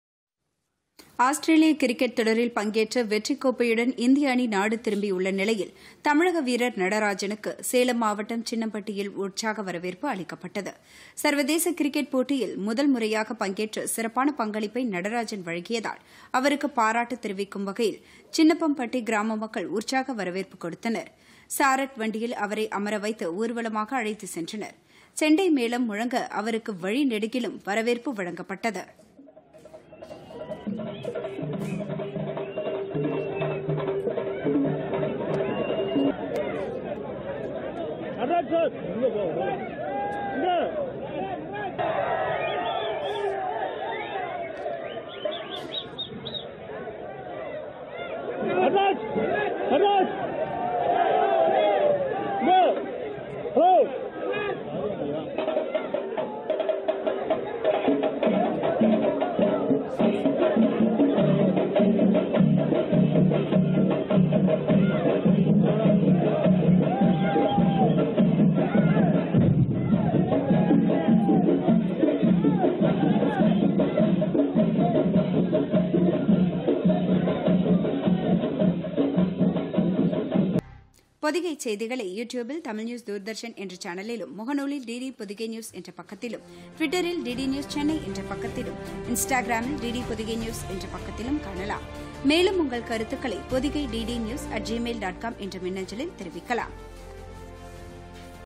கிரிக்கெட் வீரர் நடராஜனுக்கு உற்சாக வரவேற்புPodhigaiTamilNews
ஆஸ்திரேலியா டெஸ்ட் தொடர் வெற்றியுடன் சேலம் திரும்பிய நடராஜனுக்கு சொந்த ஊரில் உற்சாக வரவேற்பு - சாரட் வண்டியில் அழைத்துச் சென்று மரியாதை.